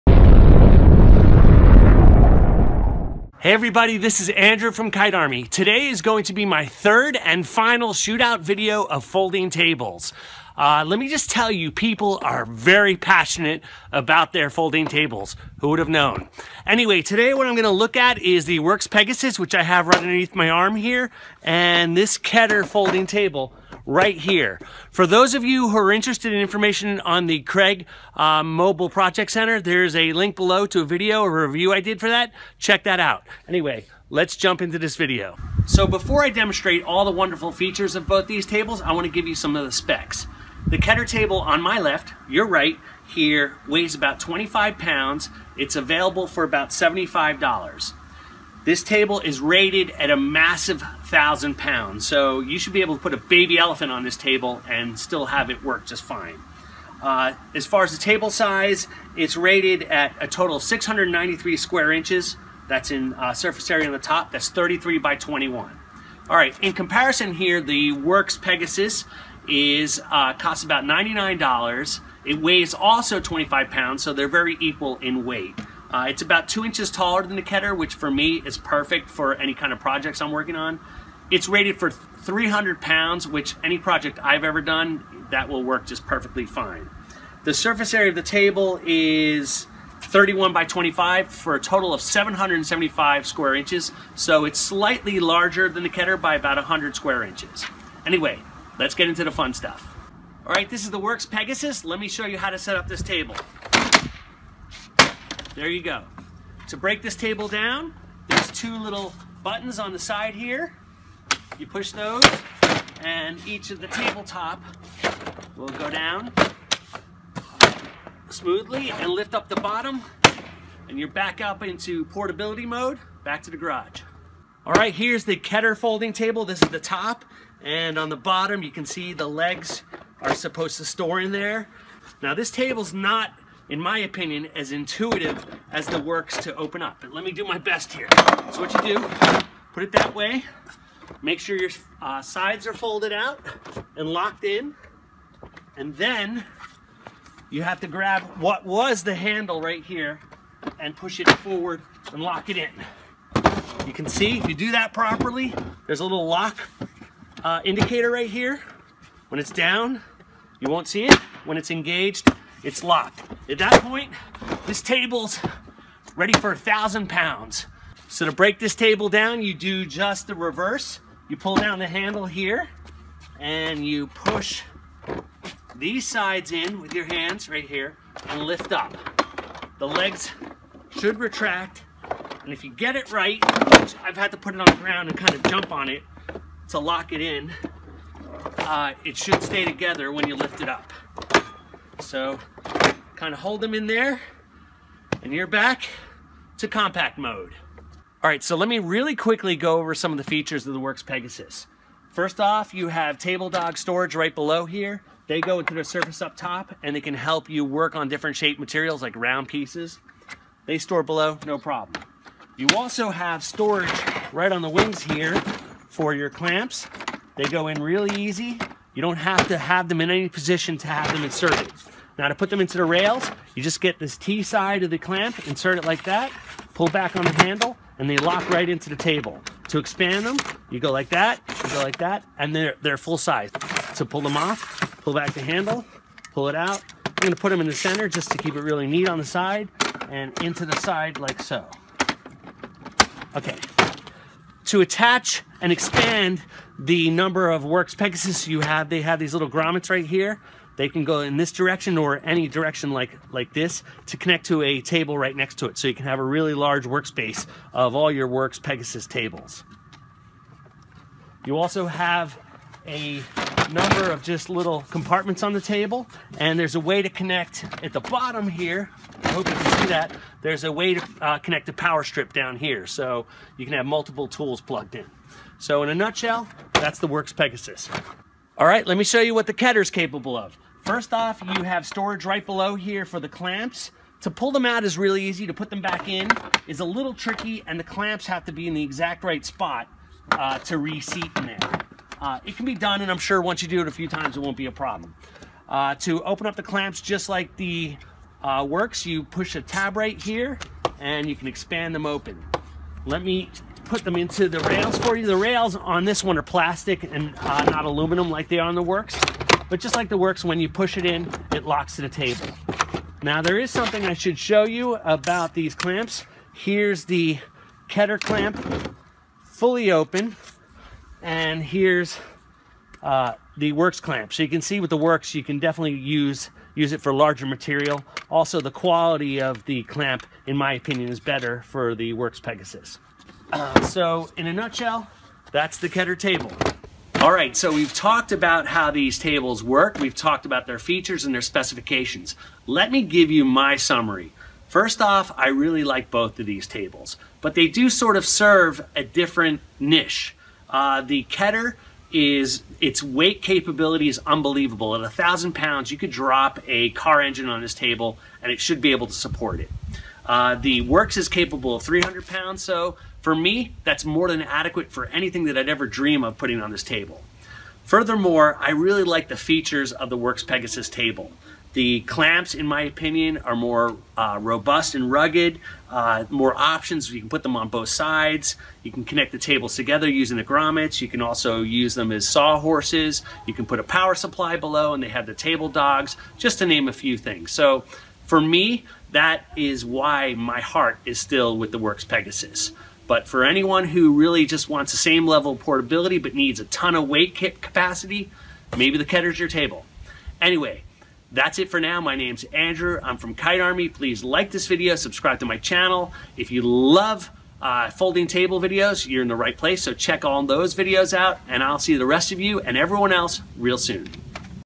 Worx Pegasus VS Keter Folding Work Table | Review and Demo 🔨🔨🔨
LINK TO BUY WORX (BEST DEAL)
LINK TO BUY KETER (BEST DEAL)

This is the Superbowl of folding work tables.  One can hold 1/2ton and the other is a garage ninja.  Enjoy!
Worx Pegasus vs Keter shop tables

 home depot lowes workspace keter sams club clamp